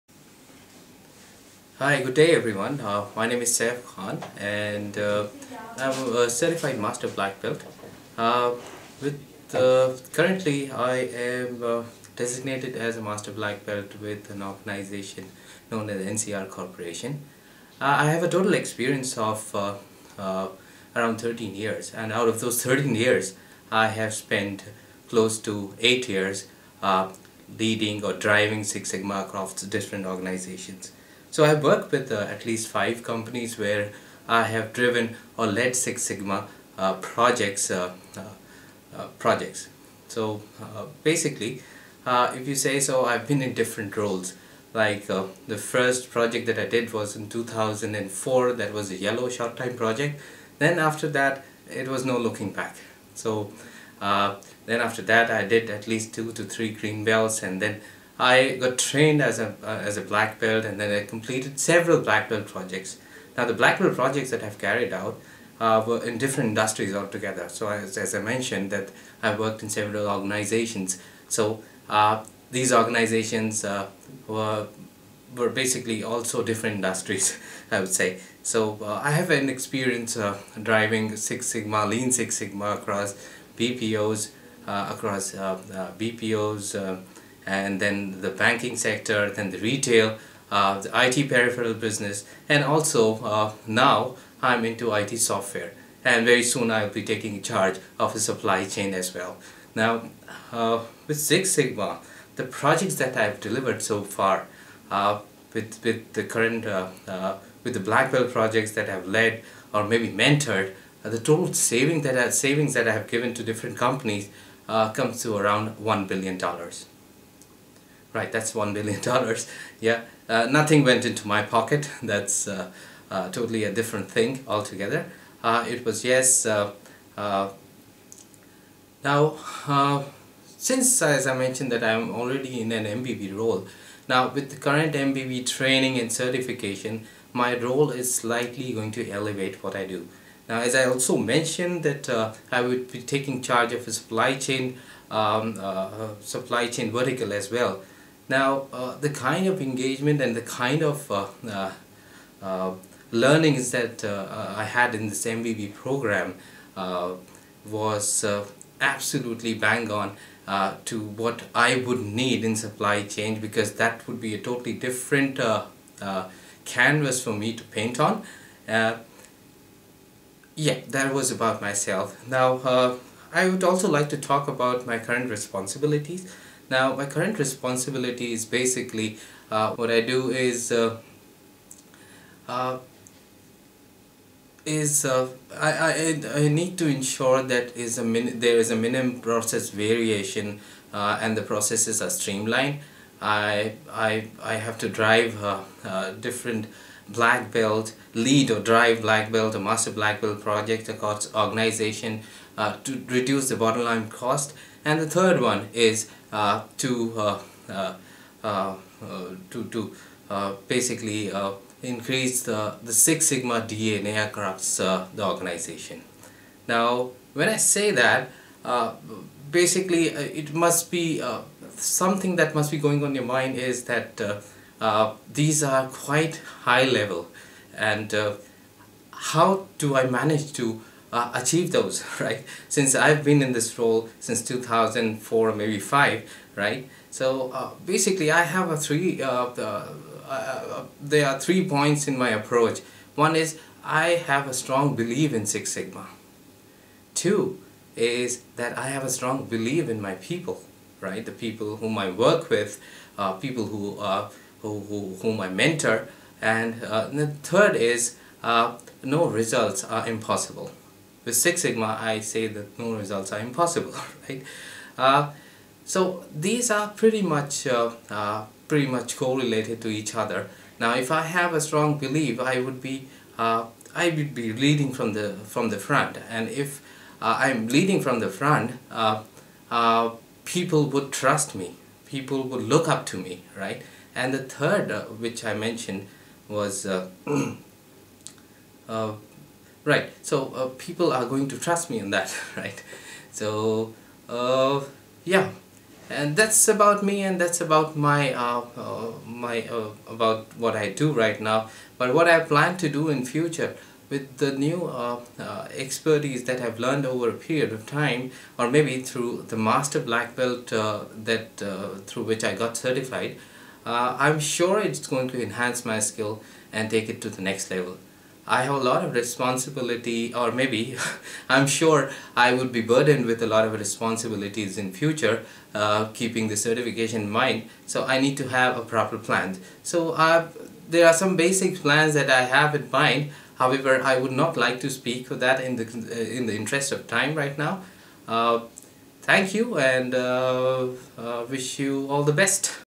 Lean Six Sigma in IT - Saif khan, MBB, NCR Corporation
Saif Khan, who, in his 13 years experience, has worked in various domains including BPO, Banking, Retail, IT Peripheral Business, IT Software, and Supply Chain Management highlights the savings he has brought.